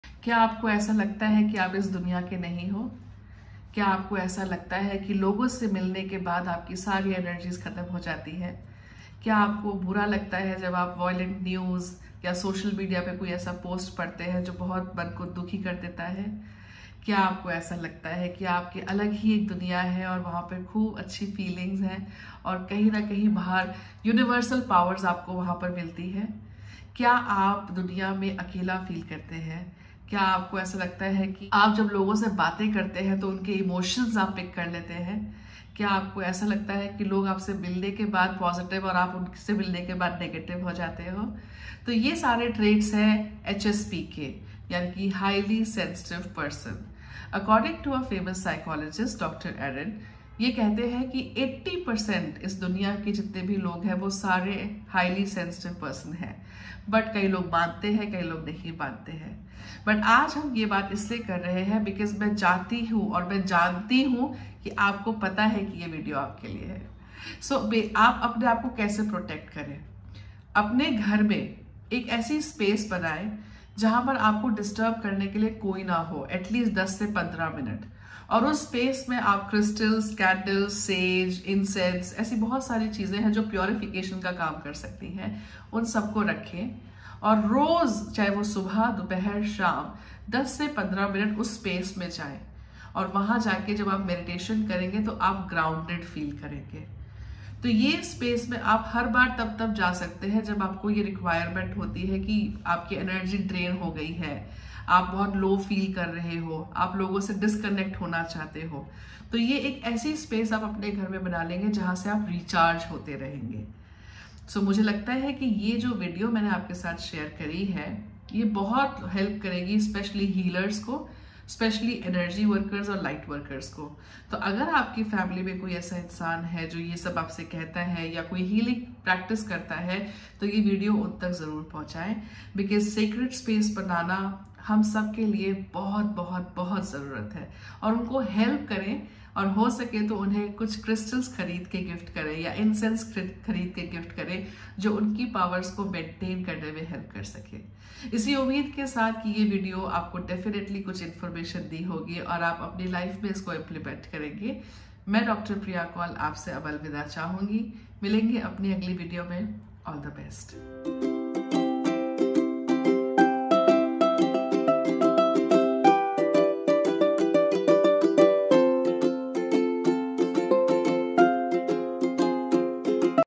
कहीं आप एक HSP तो नहीं? || Signs that you are a HSP || Traits of a HSP || Who is a HSP
Watch this video to know if you are a HSP